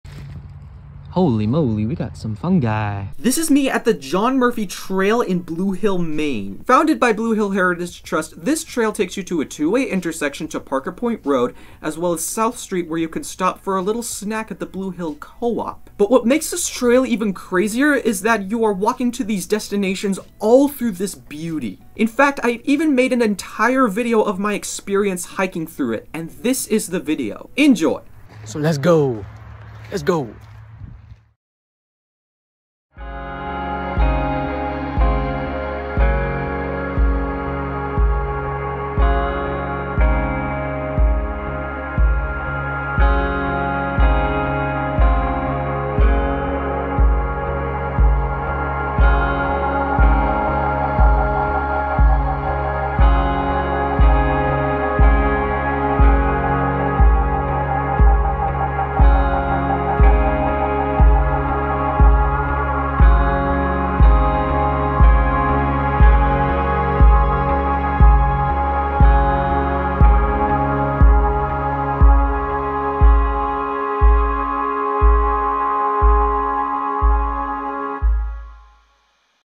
John Murphy & South Street Parker Point Trail- Blue Hill Heritage Trust 1 Min Video by Quinn Jackson
Join Quinn Jackson for a fun tour of Blue Hill Heritage Trust's John Murphy & South Street Parker Point Trail, in the Village of Blue Hill. Quinn, at the time of this recording (2023), is a Jr at George Stevens Academy and made this video for BHHT as a part of his volunteer requirement for school and towards his Eagle Scout rank in the Boy Scouts. Thanks Quinn!